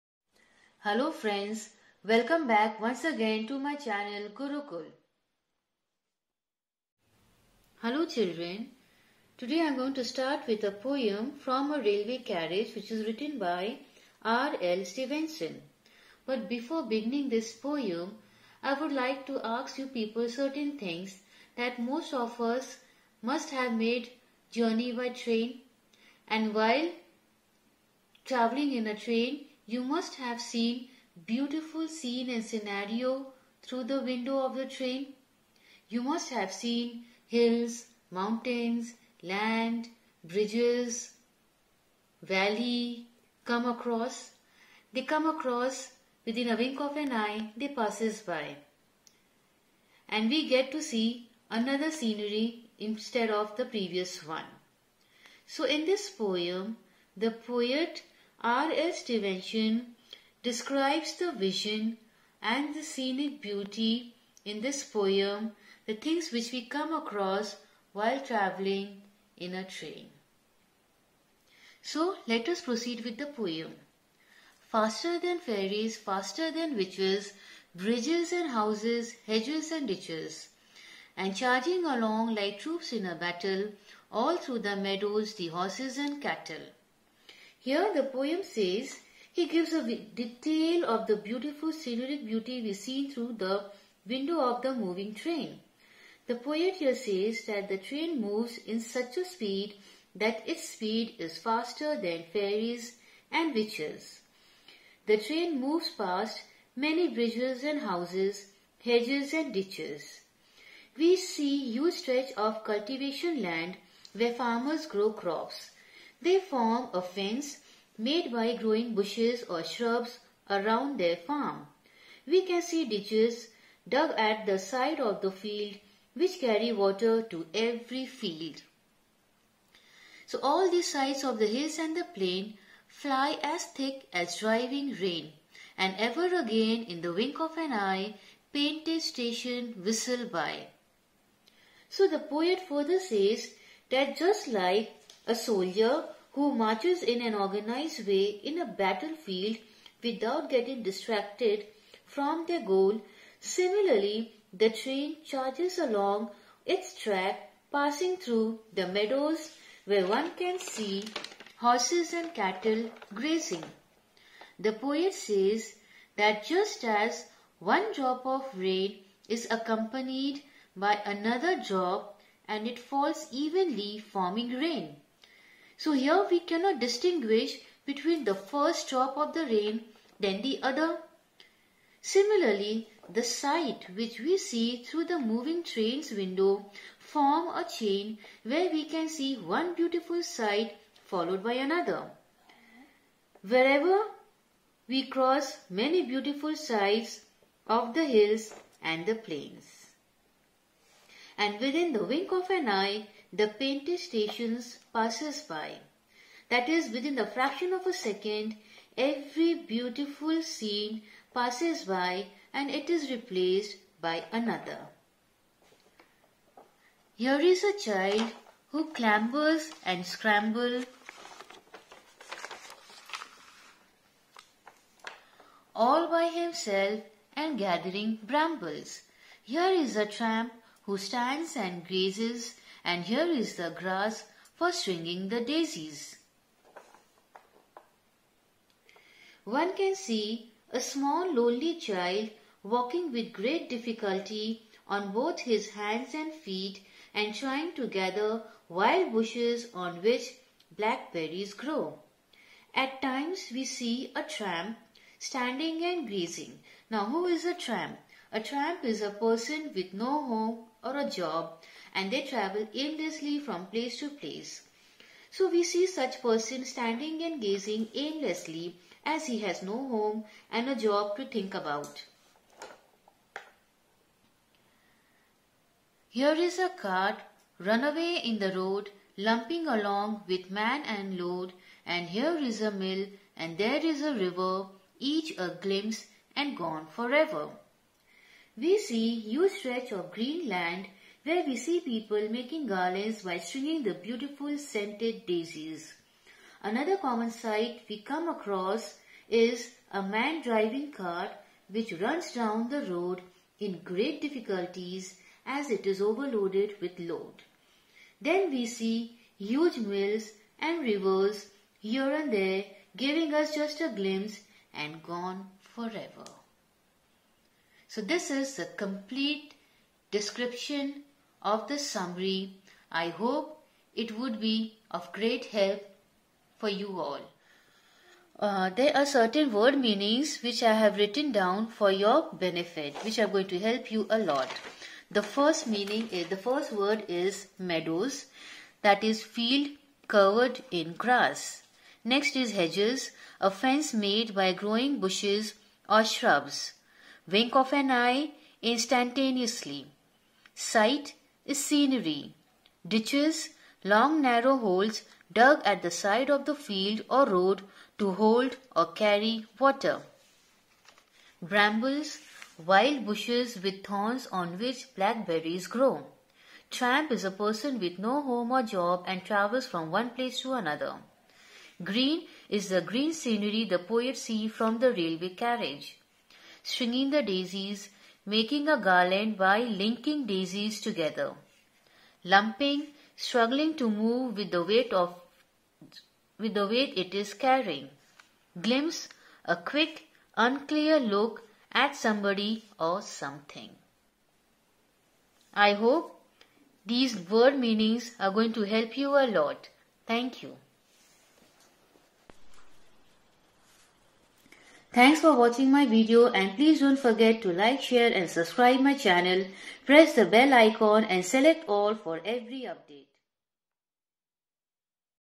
"From A Railway Carriage" Written by - R.L. Stevenson.
Summary, explanations and meanings of the poem.